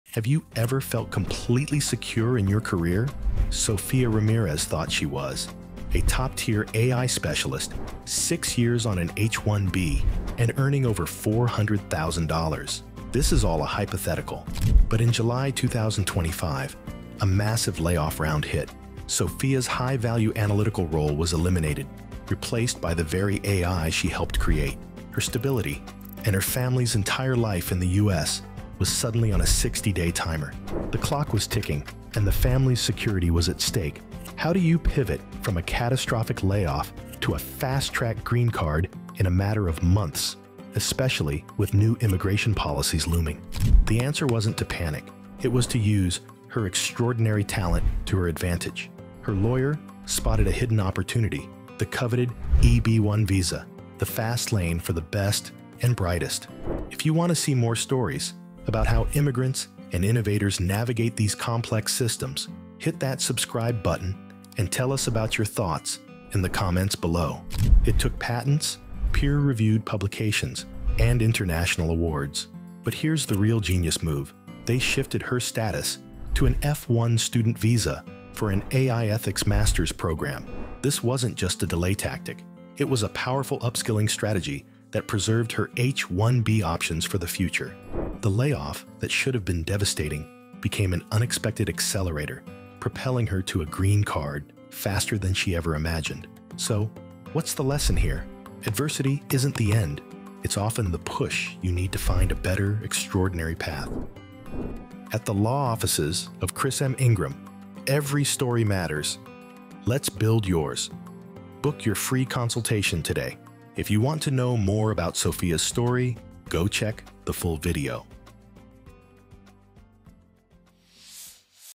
From $400K to Uncertain Future — How Sofia Secured Her Green Card
Sofia had it all: $400K salary, 6 years on H-1B, family secure.
Then AI replaced her role — 60-day grace period started.
Panic? No — her lawyer saw extraordinary ability:

Patents, peer-reviewed publications, international awards
Pivoted to F-1 for AI ethics master’s — upskilled + preserved options
EB-1A approved — green card faster than ever

Attorney Chris M. Ingram:
“Layoffs aren’t endings — they’re accelerators for the truly extraordinary.”
Your expertise can be your permanent future — no employer needed.

*Host's voice generated with AI.